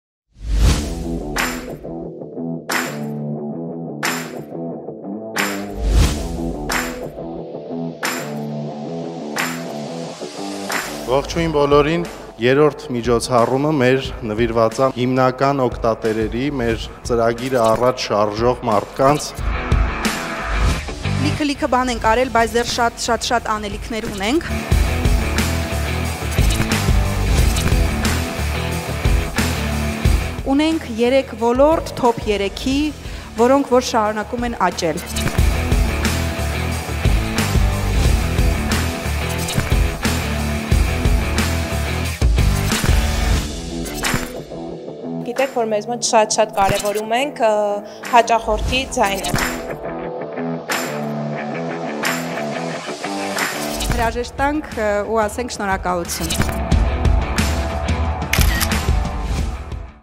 StaffAm HR GALA EVENT
StaffAm HR GALA EVENT միջոցառմանը իրենց մասնակցությունն ունեցան ավելի քան 100 ընկերությունների HR մասնագետներ։ 🔻Միջոցառումն անցավ տաք և անակնկալներով լի մթնոլորտում։ Ծրագիրը հագեցած էր staff.am նոր պրոդուկտների, Գործատուի բրենդավորման staff.am մոտեցման, ՀՀ աշխատաշուկայի վիճակագրական վերլուծության մասին ելույթներով, հաճելի երաժշտությամբ, HR-ին նվիրված հումորային StandUp-ով, յոգայի ընդմիջմամբ, Wine&Cheese հյուրասիրությամբ․ ահա այն ամենը, ինչն ապահովեց օրվա բարձր տրամադրությունը։